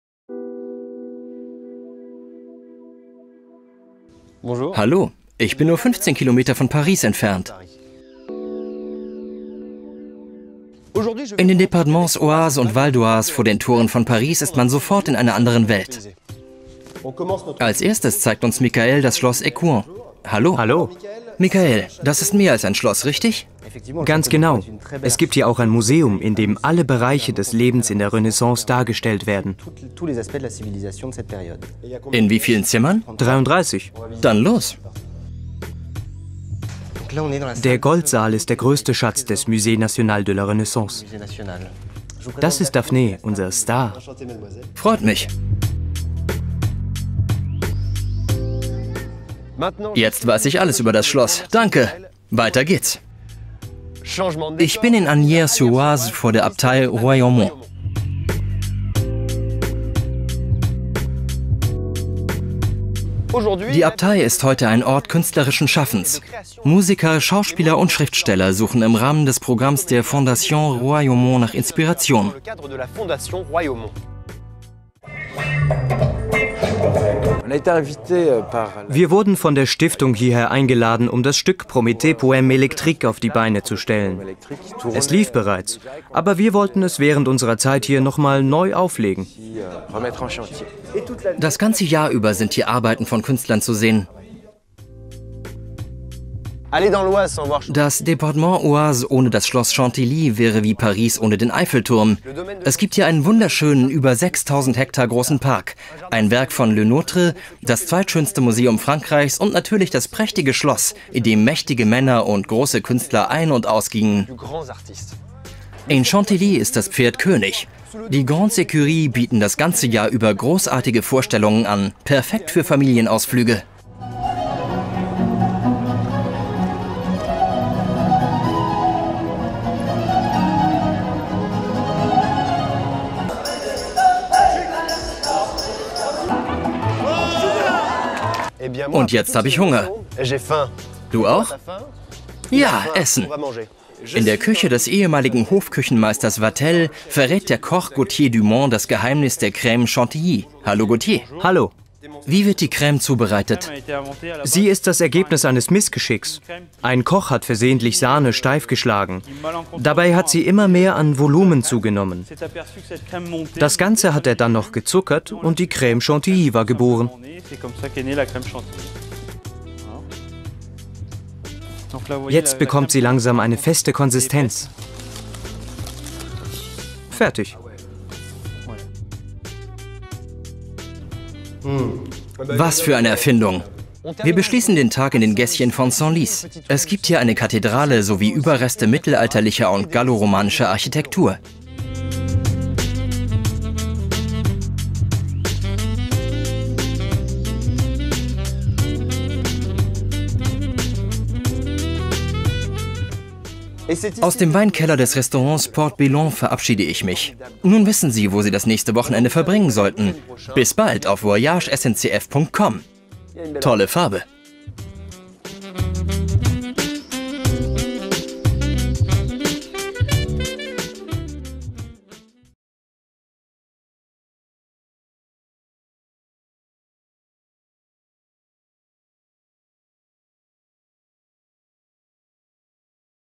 Oise - eine besondere Tour
Nur 15 Kilometer von Paris entfernt und schon eine ganz andere Welt. In Oise taucht man ab in eine längst vergessene Zeit. Entdecken Sie alte Schlösser und wunderschöne Landschaften.